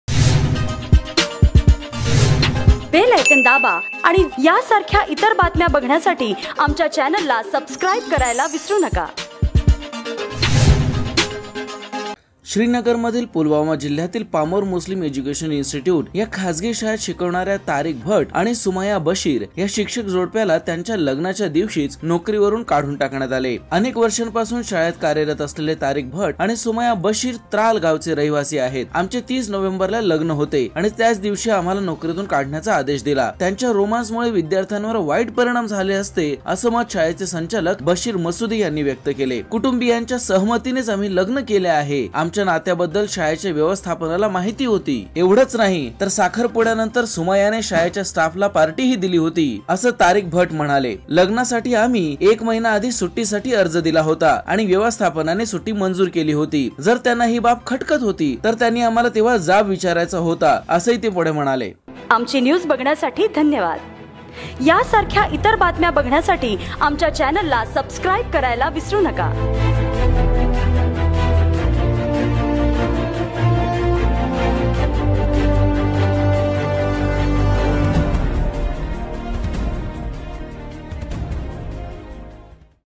प्रेम विवाहामुळे गेली नोकरी | पाहा हा वीडियो | Lokmat News
श्रीनगर मधील पुलवामा जिल्ह्यातील पाम्पोर मुस्लिम एजुकेशन इन्स्टिटयूट ह्या खासगी शाळेत शिकवणाऱ्या तारिक भट आणि सुमाया बशीर ह्या शिक्षक जोडप्याला त्यांचा लग्नाचा दिवशीच नोकरीवरून काढून टाकण्यात आले. अनेक वर्षांपासून शाळेत कार्यरत असलेले तारिक भट आणि सुमाया बशीर त्राल गावचे रहिवासी आहेत. आमचे 30 नोव्हेंबरला लग्न होते आणि त्याच दिवशी आम्हाला नोकरीतून काढण्याचा आदेश दिला .त्यांच्या रोमान्स मुळे विद्यार्थ्यांवर वाईट परिणाम झाले असते अस मत शाळेचे संचालक बशीर मसूदी ह्यांनी व्यक्त केले.कुटुंबीयांचा सहमतीनेच आम्ही लग्न केले आहे . आमच्या नात्याबद्दल शाळेच्या व्यवस्थापनाला माहिती होती. एवढंच नाही तर साखरपुड्यानंतर सुमायाने शाळेच्या स्टाफला पार्टीही दिली होती असं तारिक भट म्हणाले. लग्नासाठी आम्ही एक महिना आधी सुट्टी साठी अर्ज दिला होता आणि व्यवस्थापनाने सुट्टी मंजूर हि केली होती, जर त्यांना हि बाब खटकत होती तर त्यांनी आम्हाला तेव्हाच जाब विचारायचा होता असेही ते पुढे म्हणाले.